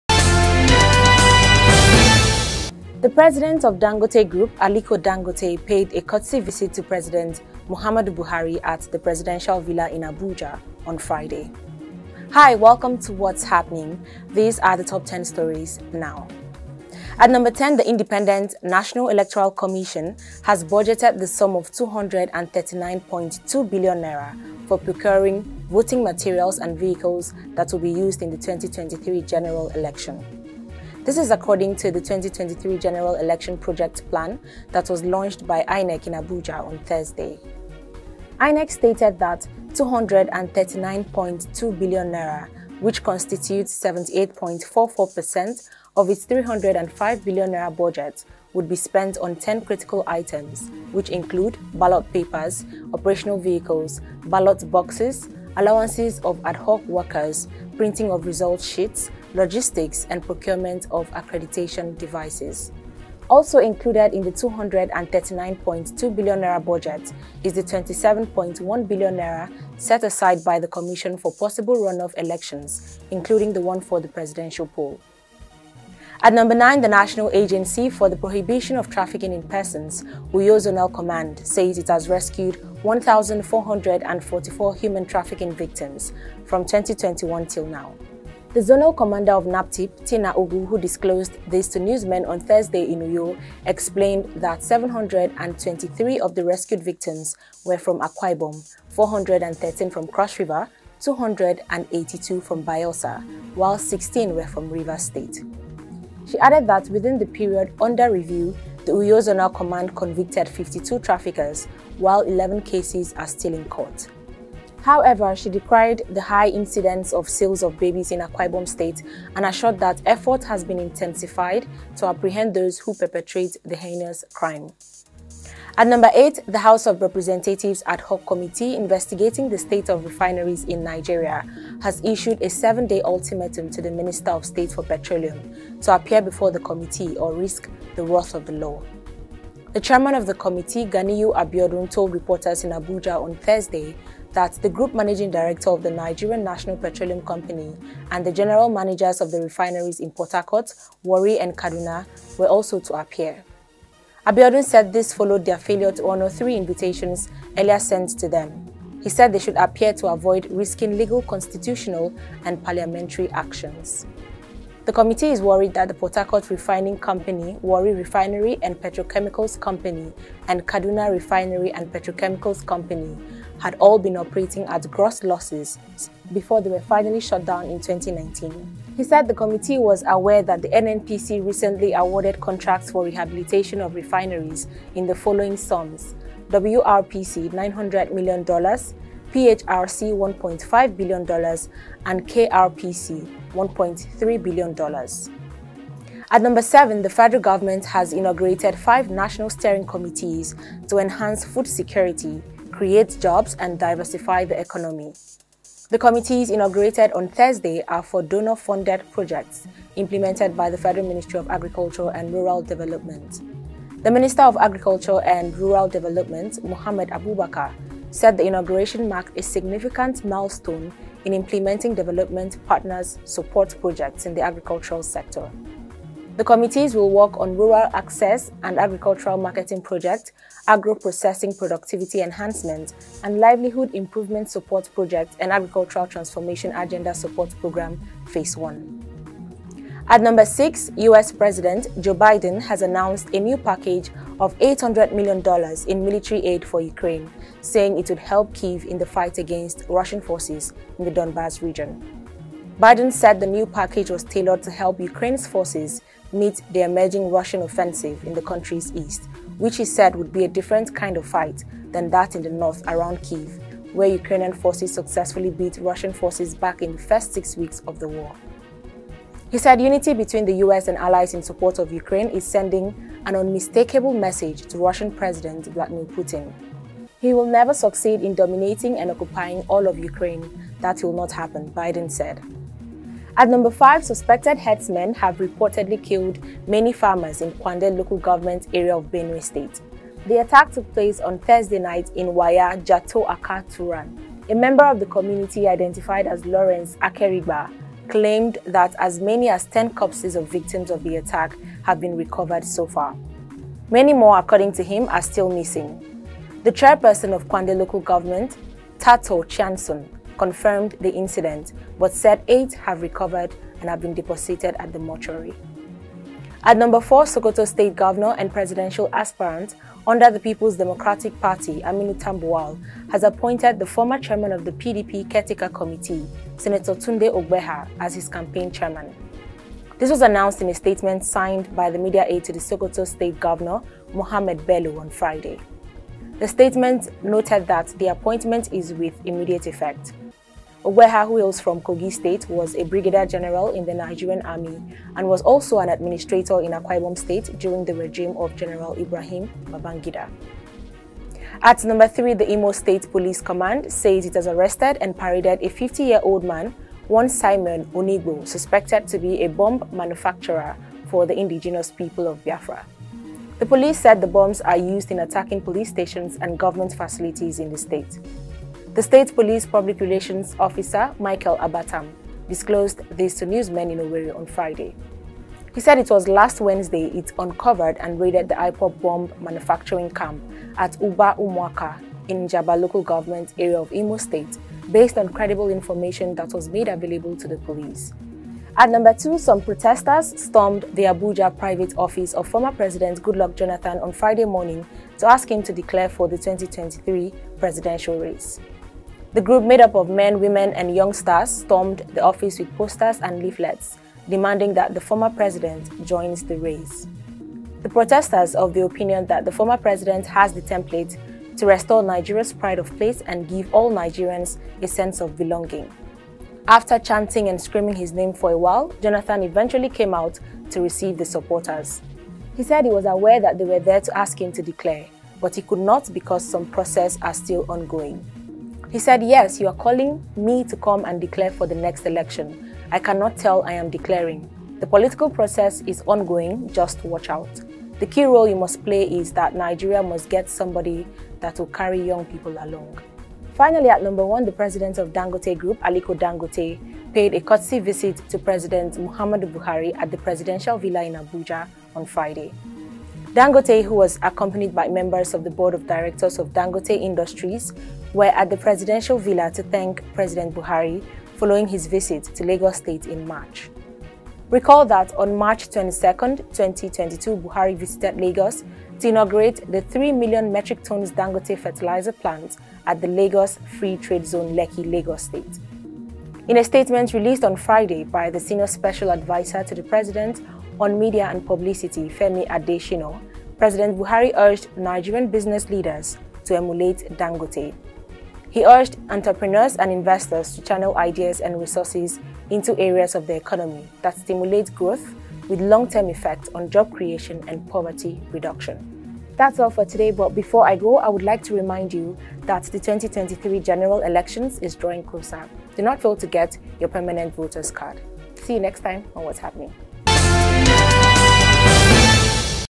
Dangote visits Buhari in Aso Rock
The President of Dangote Group, Aliko Dangote, paid a courtesy visit to President Muhammadu Buhari at the Presidential Villa in Abuja on Friday.